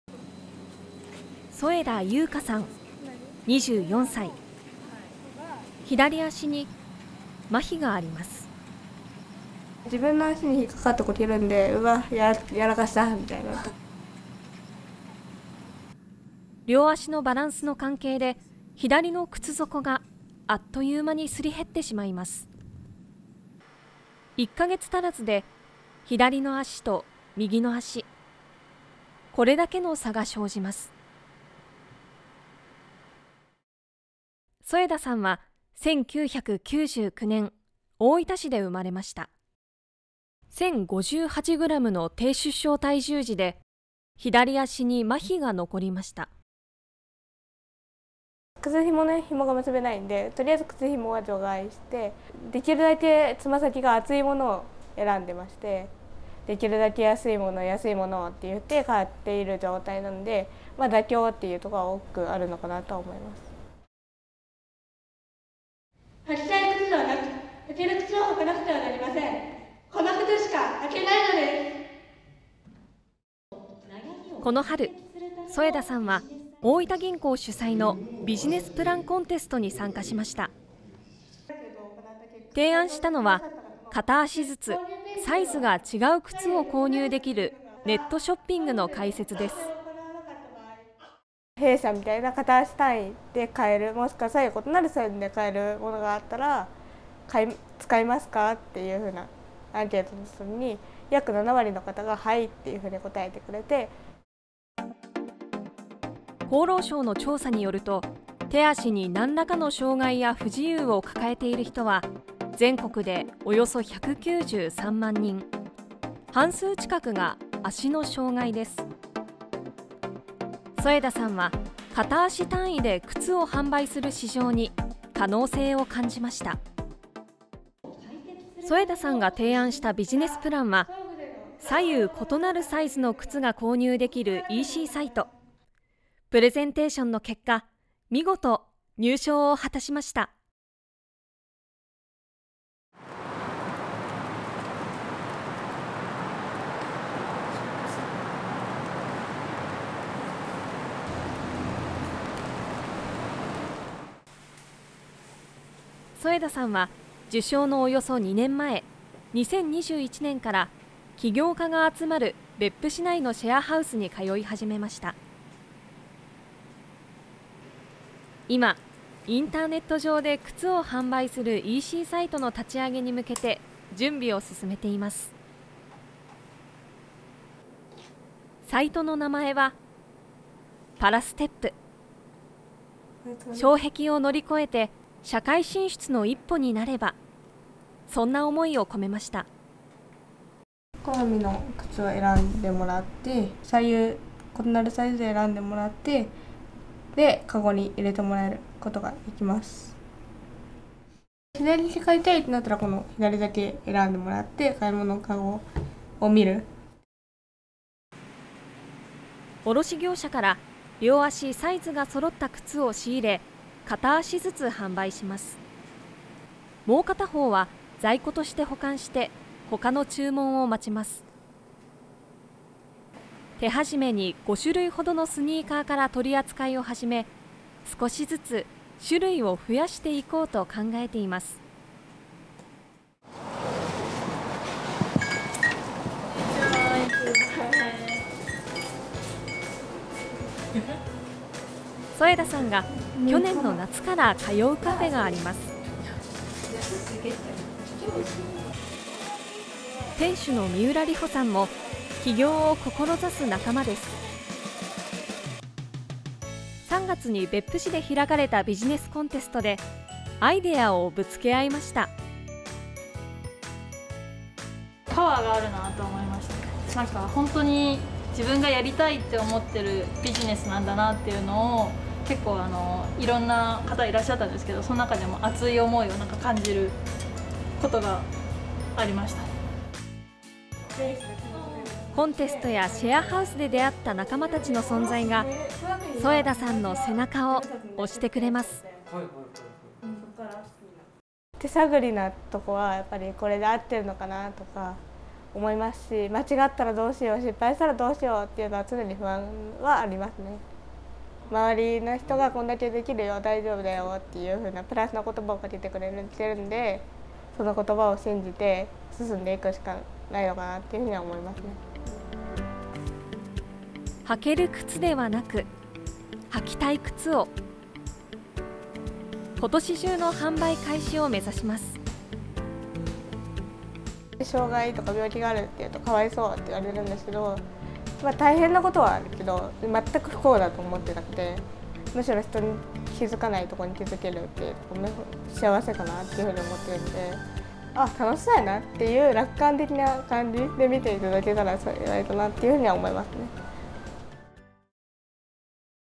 【特集】履きたい靴を履ける世界に
大分市の女性が、ある、ネットショッピングを立ち上げようと奮闘しています。
その挑戦の裏にあるのは自分自身の経験です。